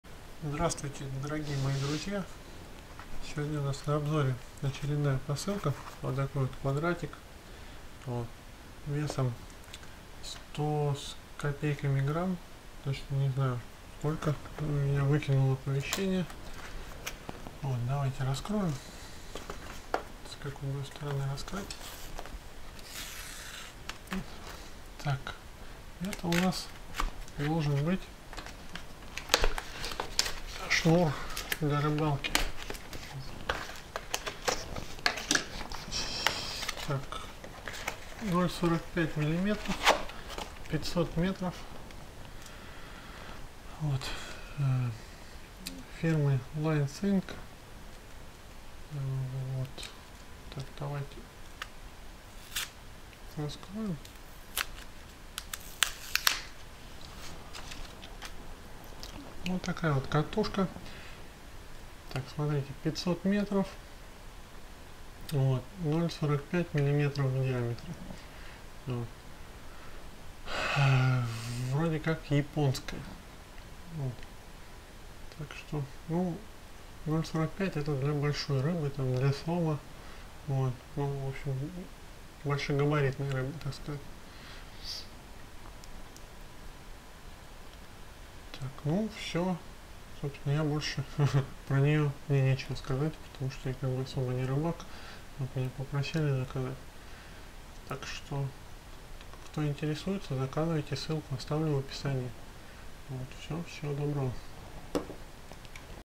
Китайский шнур для спиннинга LineThink
Ничего особо в рыбалке не понимаю, единственное выяснил что он выдерживает до 45,5 кг веса.